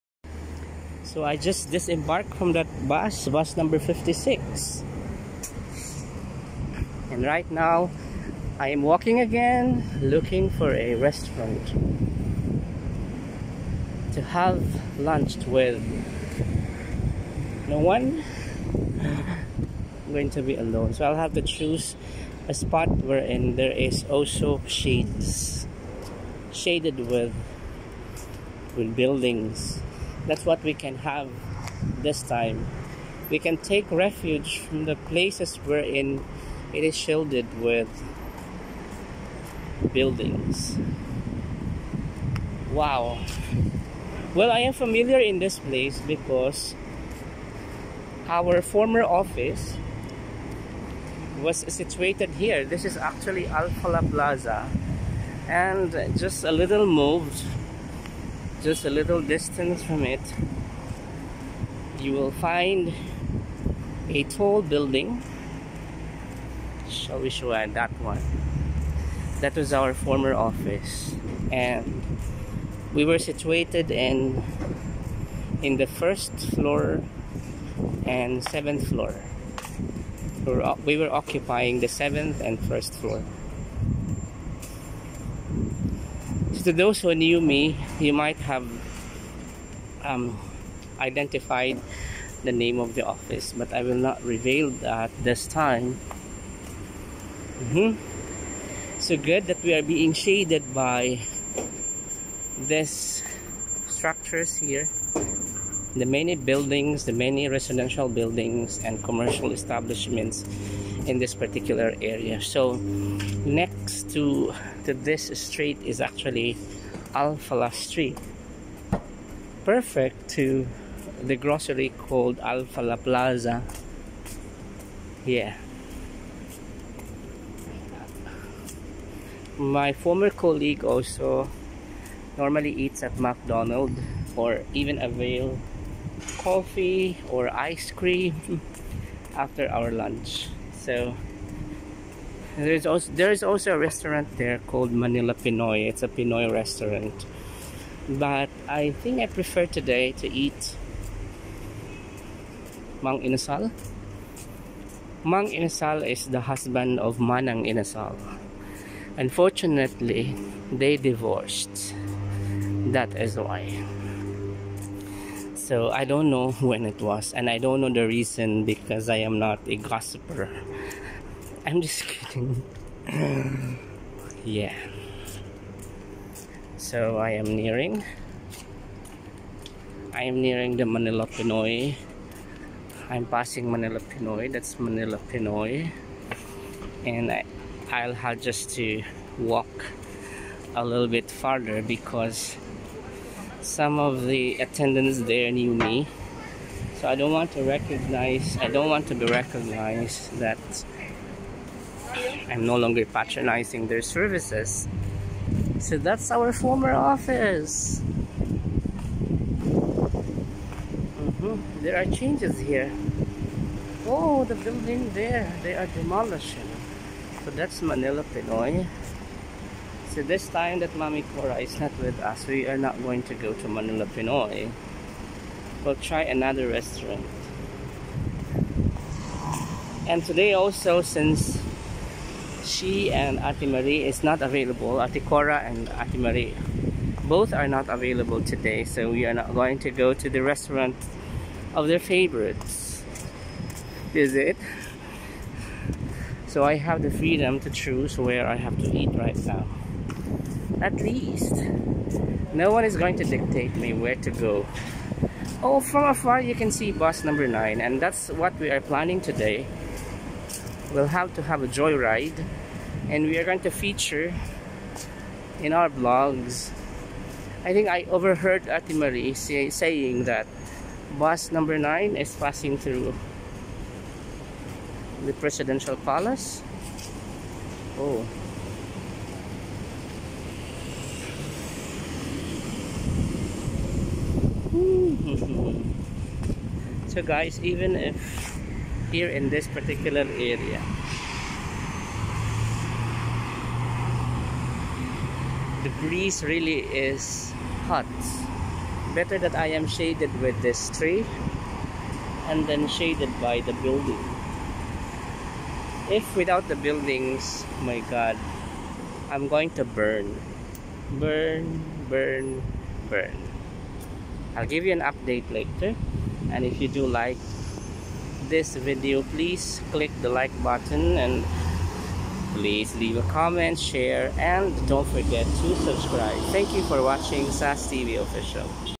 The Heat is On | SAS TV Official
Reminiscing my Al Falah days in the middle of a very hot temperature.